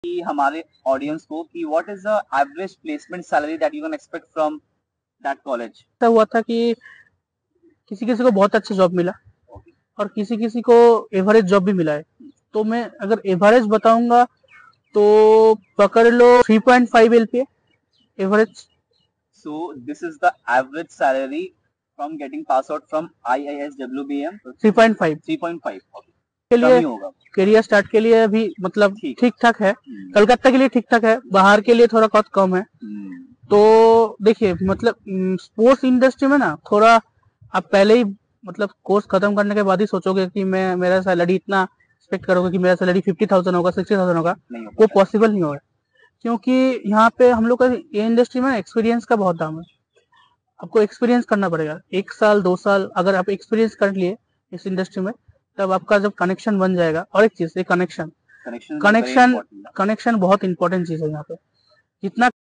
WHAT WILL BE THE SALARY ??? WHY CONNECTION IS IMPORTANT 👀😰
Discuss for a very important topic. That is what will be the salary amount that we would be getting after competing Sports Management.